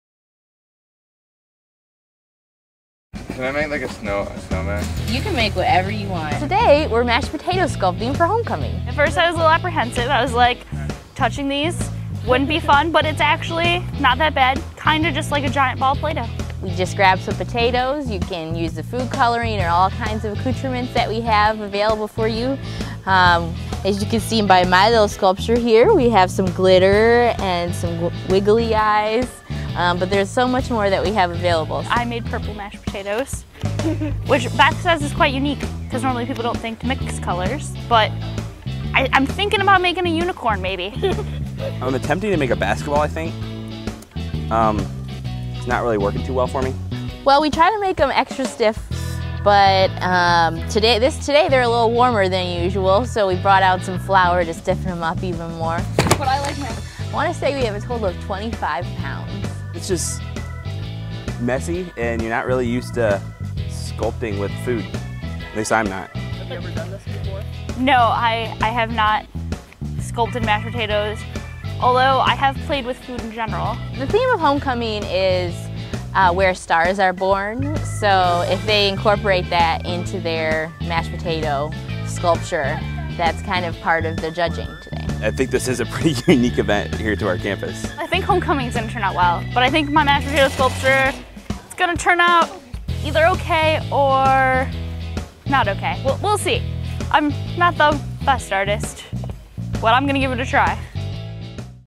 Mashed Potato Sculpting a tradition at UIS Homecoming
UIS students rolled up their sleeves and got messy during one of the first events celebrating Homecoming 2010.